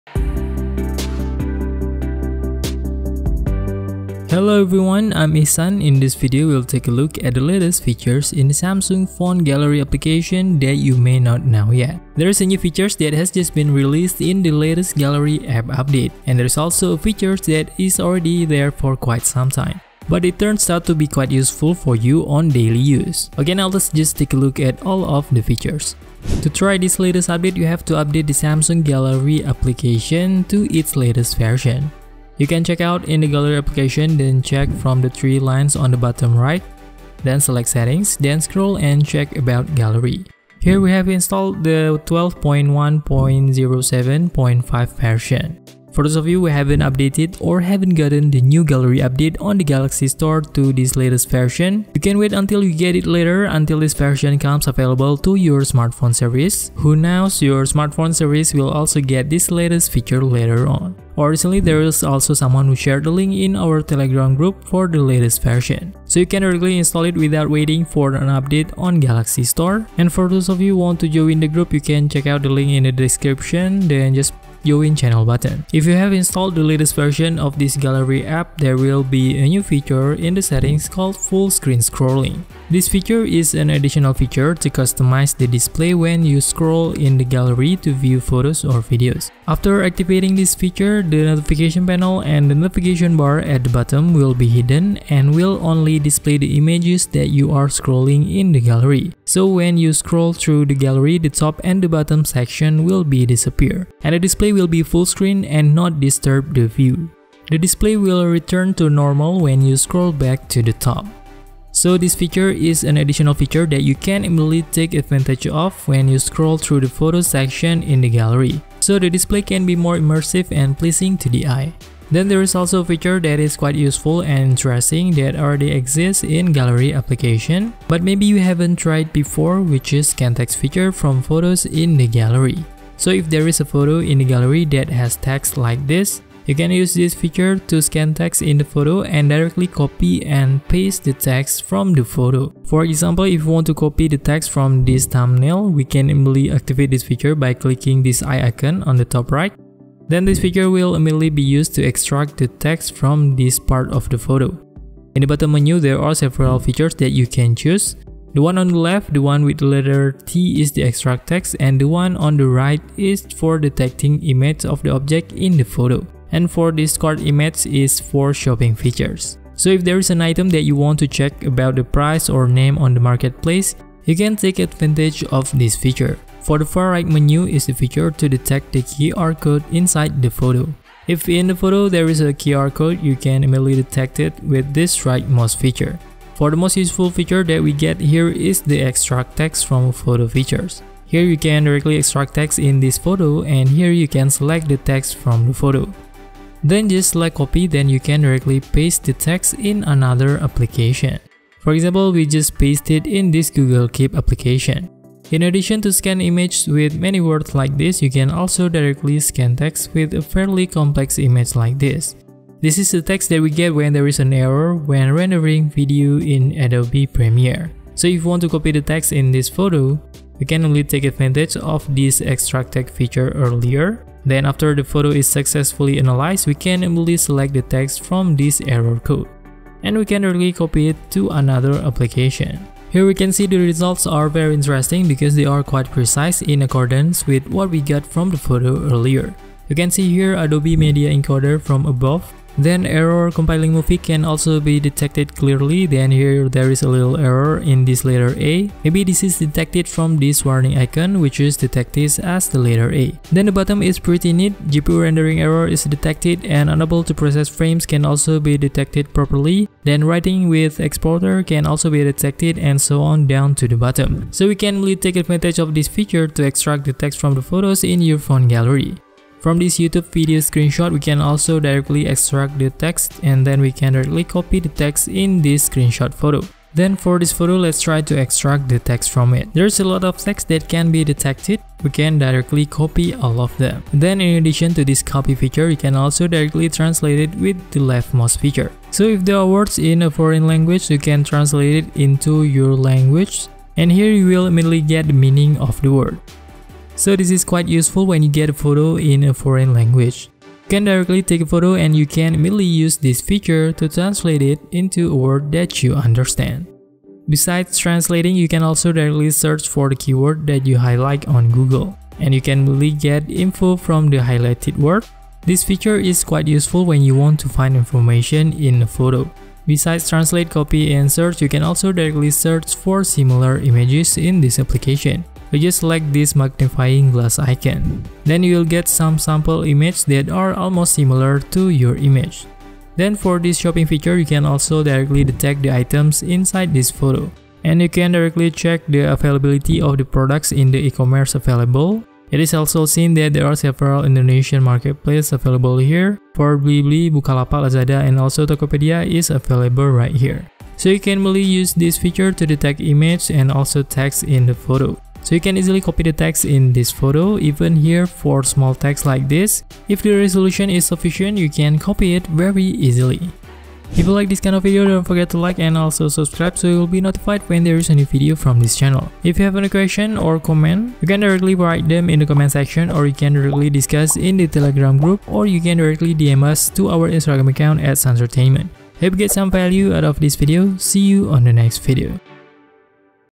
New Features in Gallery App on Samsung Phone to Copy Text Directly From Photos & New Gallery View
In this video, we will share about the latest useful features in the default gallery application of Samsung smartphones.

You can use this feature to directly scan and copy text from a photo and also detect objects in photos like on Google Lens feature.

There is also a feature in the latest update in the gallery application that we share on the video. For a more complete explanation, let's just check it out in the following video.

Join our telegram channel to get information about the latest phones, gadgets and other tips